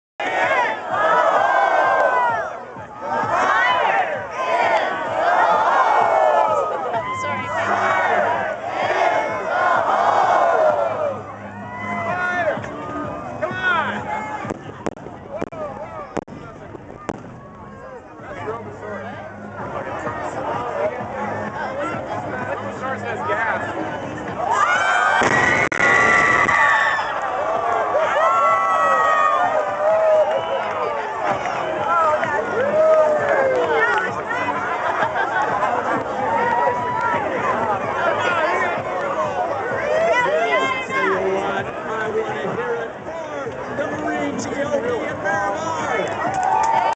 Great Wall o' Fire
The "Great Wall of Fire" event at the air show 2010 at MCAS Miramar in San Diego, California.